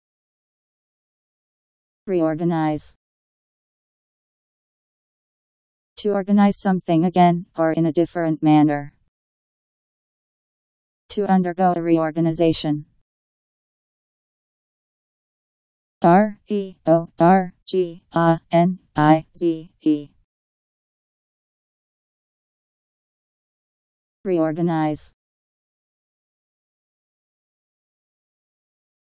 Reorganize Meaning
Video shows what reorganize means. to organize something again, or in a different manner. to undergo a reorganization.  Reorganize Meaning. How to pronounce, definition audio dictionary. How to say reorganize. Powered by MaryTTS, Wiktionary